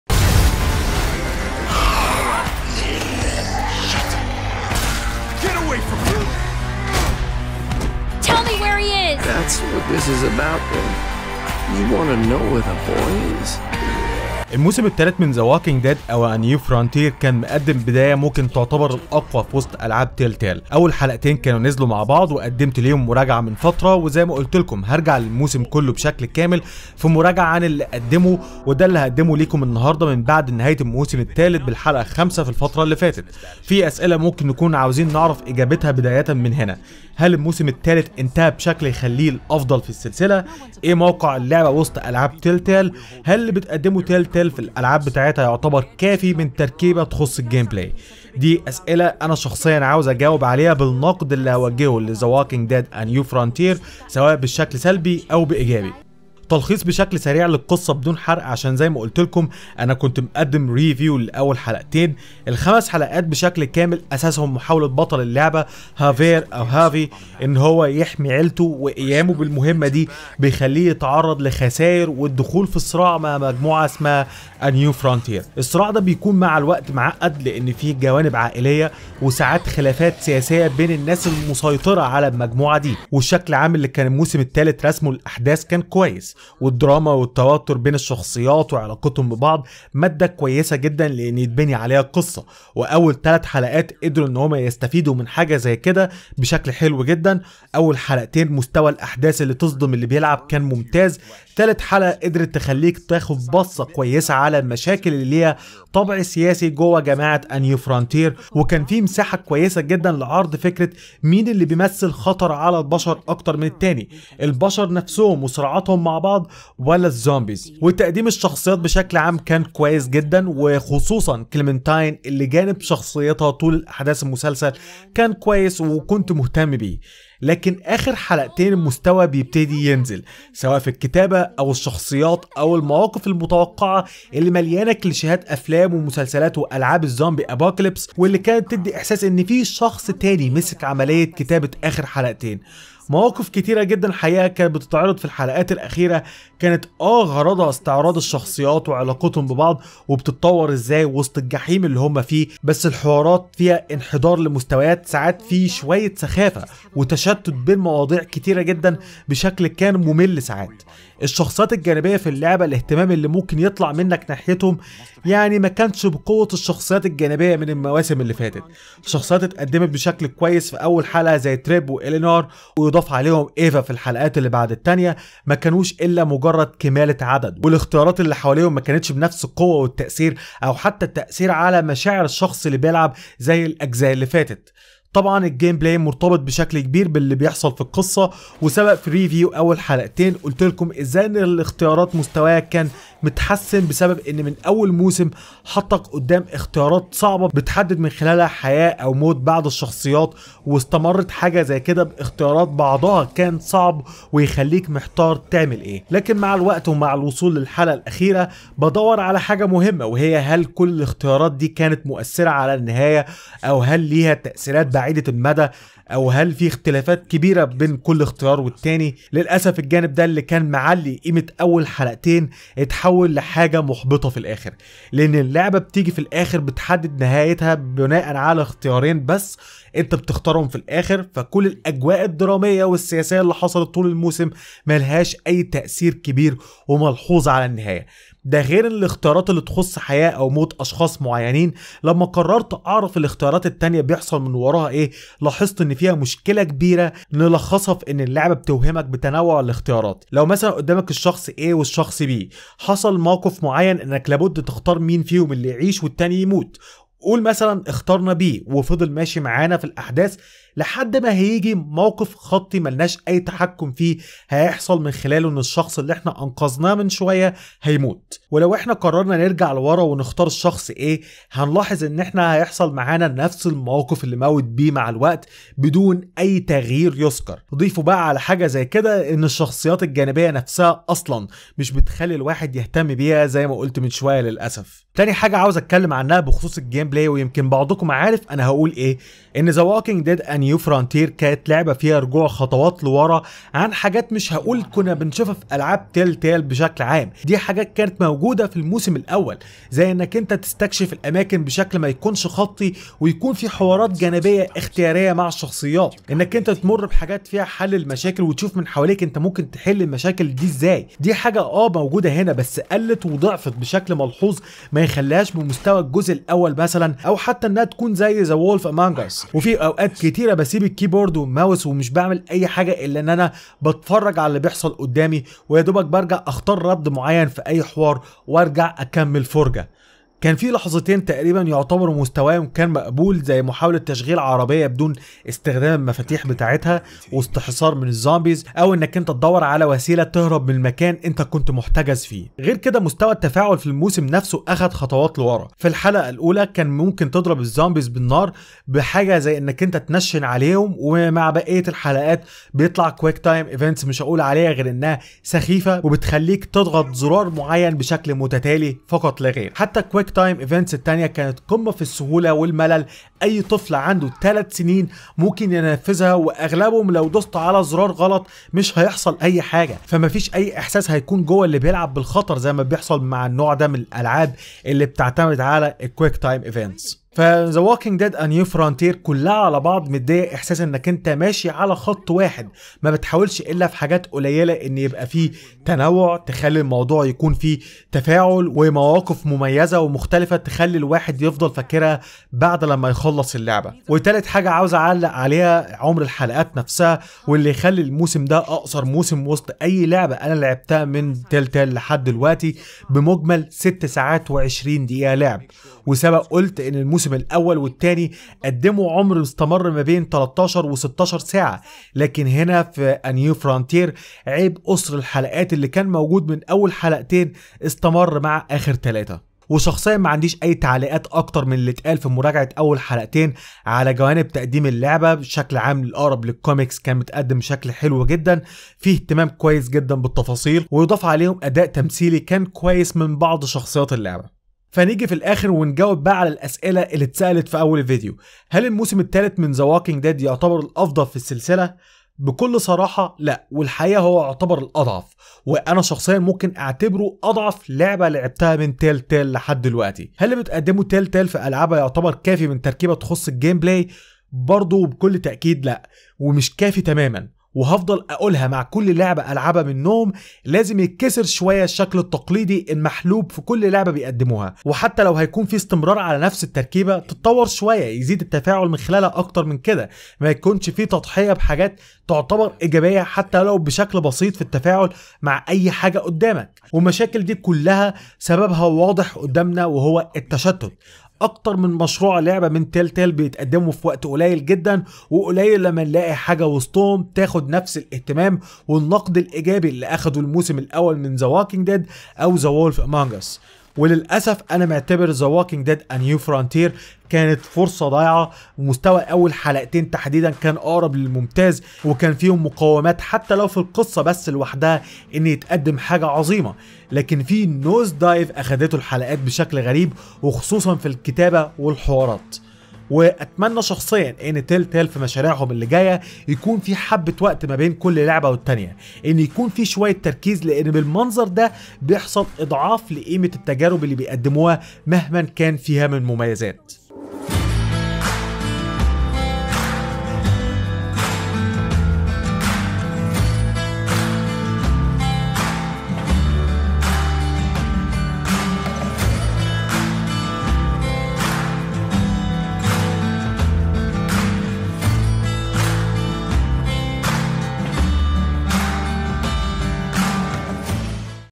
مراجعة The Walking Dead A New Frontier
مراجعة الموسم التالت من Telltale's The Walking Dead اللي تم تقديمه بأسم A New Frontier بشكل كامل بعد نهاية حلقاته بالحلقة الخامسة في الفترة اللي فاتت, المراجعة شاملة رأيي عن اللعبة سواء في القصة و تقديمها و كتابتها و مستوي التفاعل معاها من اختيارات و حوارات و ادارة المواقف اللي بتحصل طول مدة الحلقات.